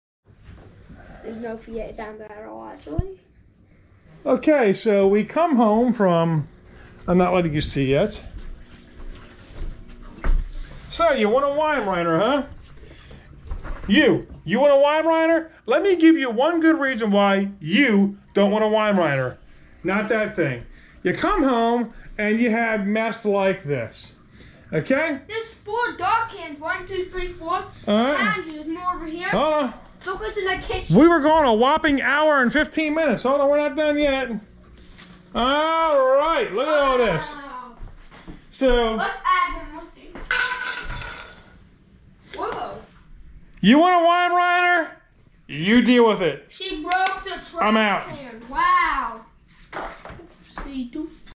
Weim Crime #4 🚀🚀🔥🔥🚀💯
Weimaraner's are GREAT DOGS IF you like your recycle cleaned!